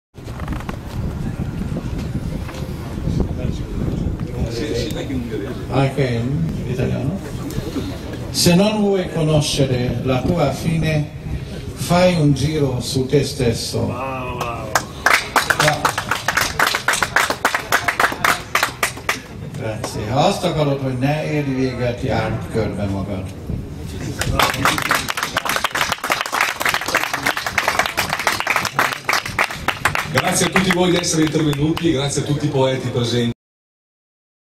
ScoglidiverSi 2011 - Géza Szőcs - 3
fluSSidiverSi | 4
Poesia e poeti di Alpe-Adria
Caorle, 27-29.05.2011

ScoglidiverSi: un verso di Géza Szőcs sulla scogliera di Caorle
Lungomare Petronia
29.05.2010